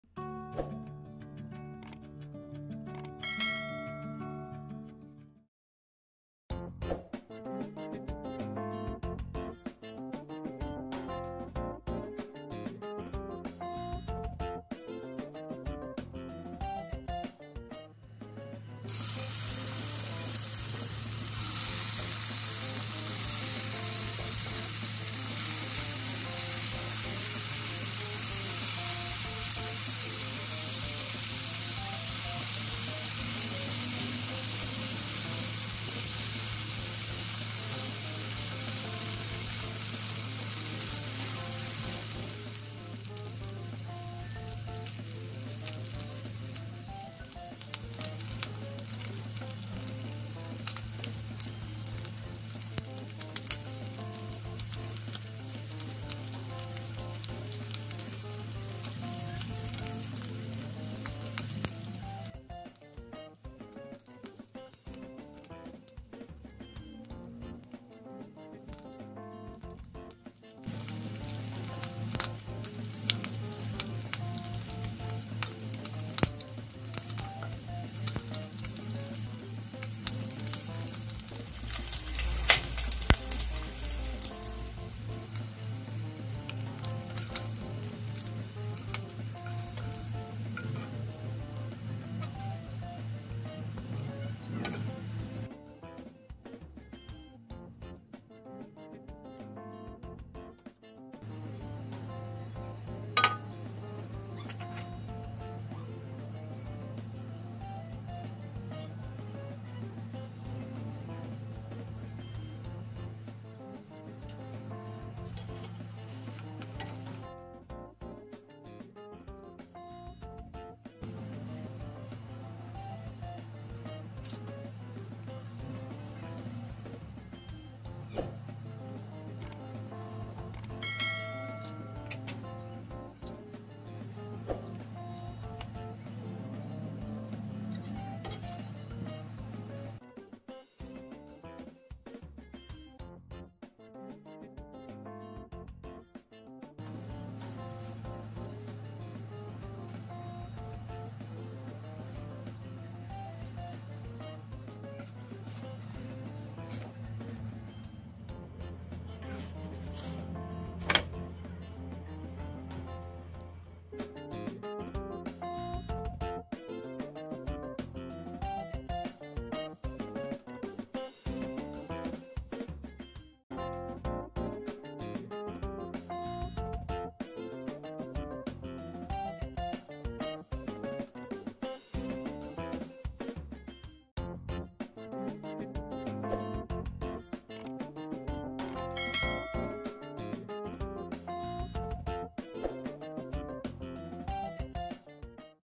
EGG SANDWICH HACK with BACON | Breakfast Sandwich | Mrs L.V.C Kitchen
My How to, VlogNo.4
Hi guys! How's everyone this quarantine period?I made a simple and easy to make sandwich hack that I've seen before, its yummy and definitely cheap for its ingredients, since that at this time we are tight on our budget 😅
Happy Cooking 😊